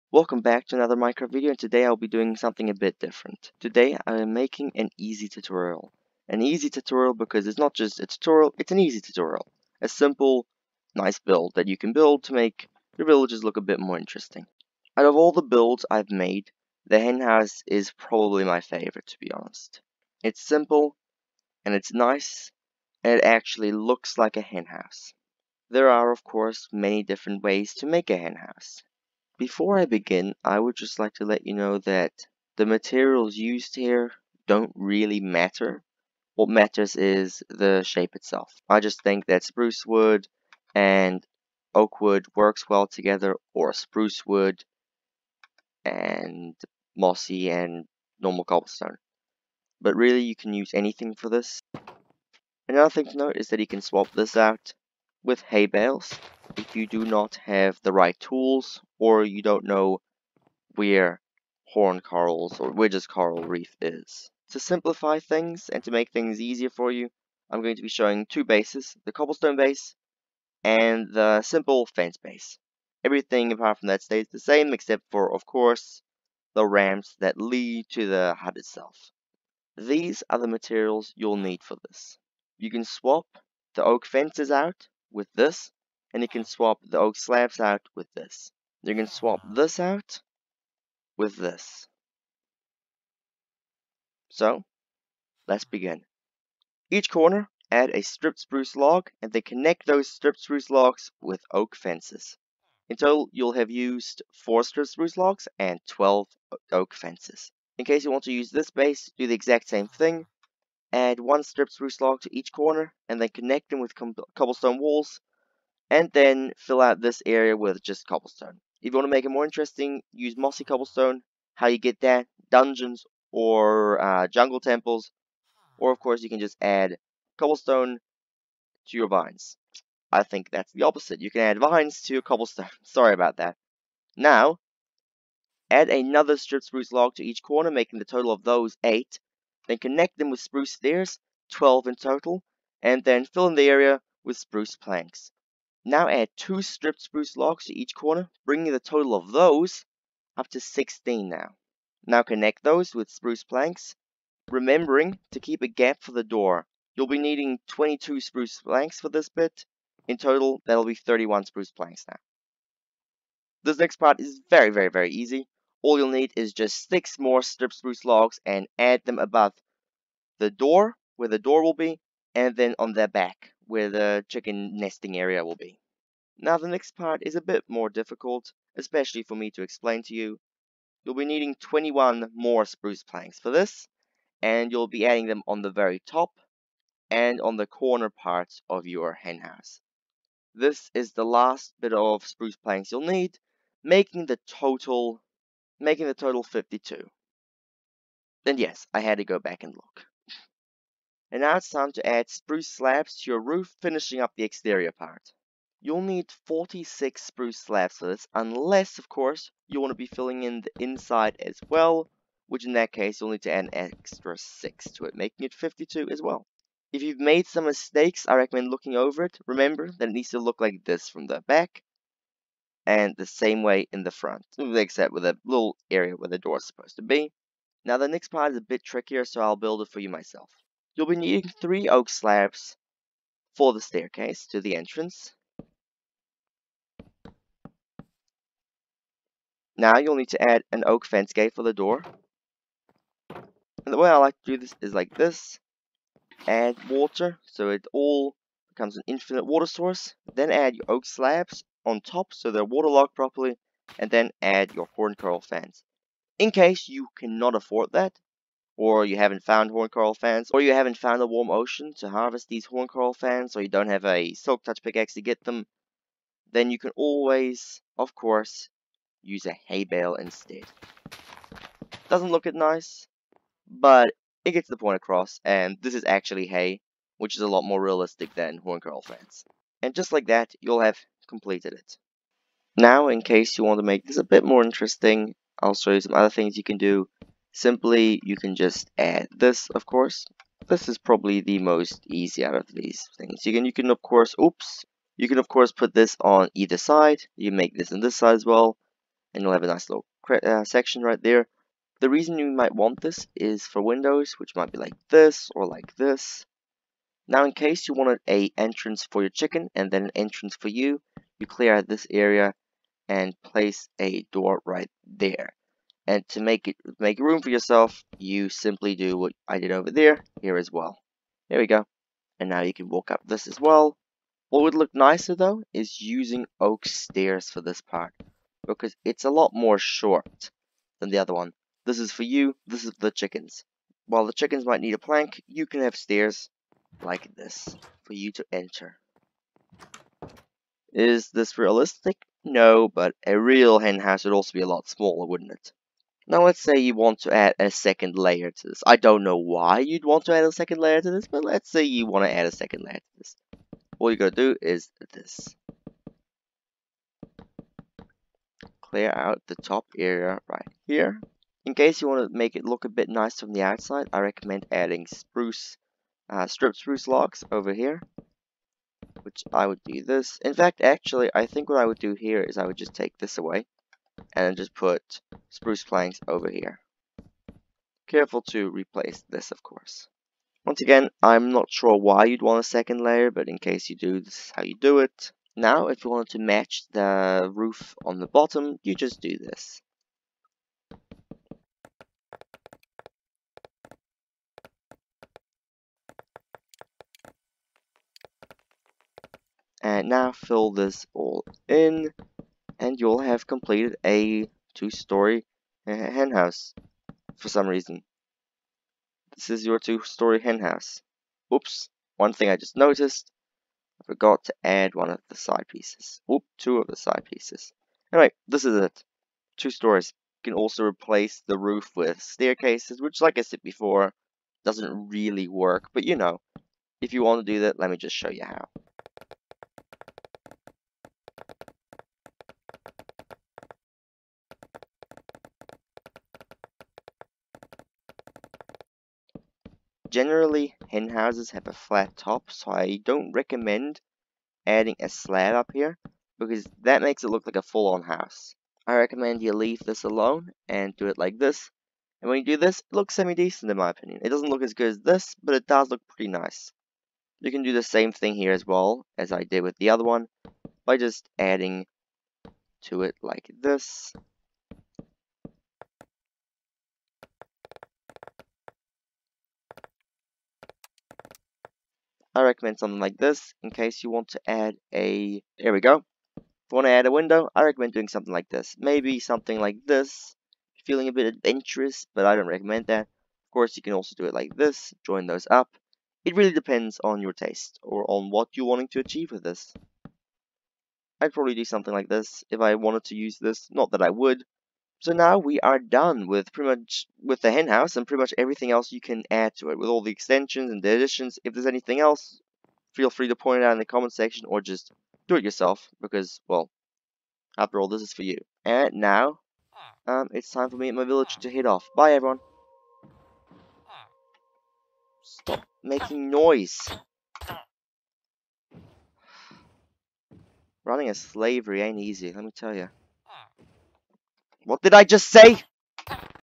Easy Tutorial - Hen House
Today I show you how to build a henhouse (my design) and some other variations of it.